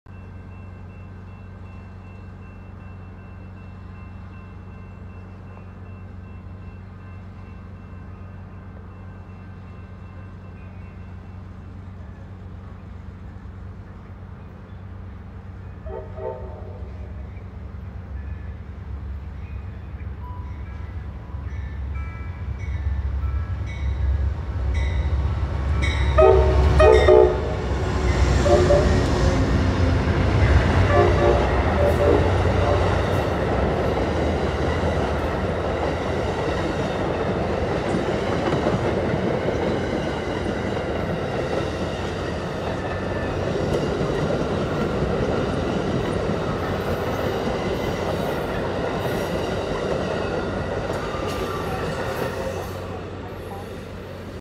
Amtrak P091 at Ashland, VA @4:50 PM
P091's Power: AMTK P42DC 83 (Leading), AMTK P42DC 10 (Trailing), AMTK P42DC 187 (Trailing)

Amtrak 91 (New York, NY - Miami, FL)

Amtrak P091 the southbound Silver Star passes Ashland Station in Ashland, VA with 3 engines running elephant style again.